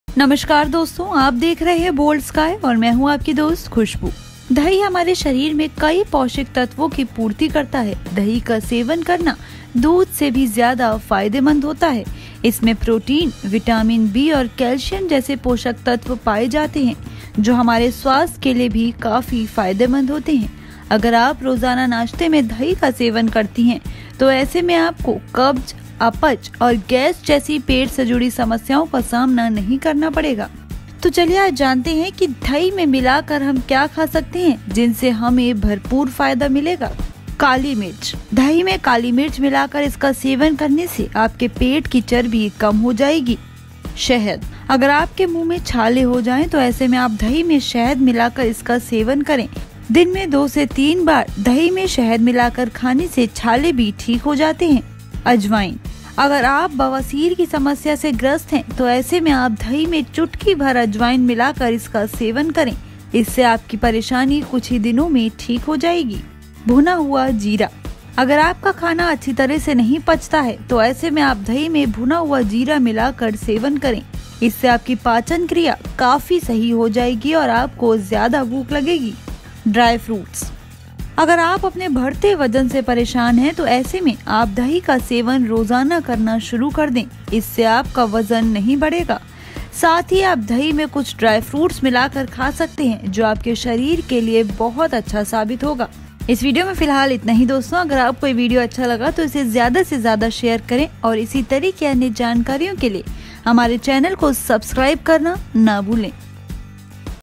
Curd में ये 5 चीज़े मिला कर जरुर खाएं , फायदे सुन कर हो जाएंगे हैरान | Curd Benefits | Boldsky
Yogurt supplies many nutritional elements in our body. Eating curd is more beneficial than milk. If you eat curd for breakfast every day, then you will not have to face stomach problems like constipation, indigestion and gas. Eat these things mixed with yogurt.

दही हमारे शरीर में कई पौषिक तत्वों की पूर्ति करता है। दही का सेवन करना दूध से भी ज्यादा फायदेमंद होता है। आप अगर रोजाना नाश्ते में दही का सेवन करती हैं, तो ऐसे में आपको कब्ज, अपच और गैस जैसी पेट से जुड़ी समस्याओं का सामना नहीं करना पड़ेगा। दही में मिलाकर खाएं ये चीजे|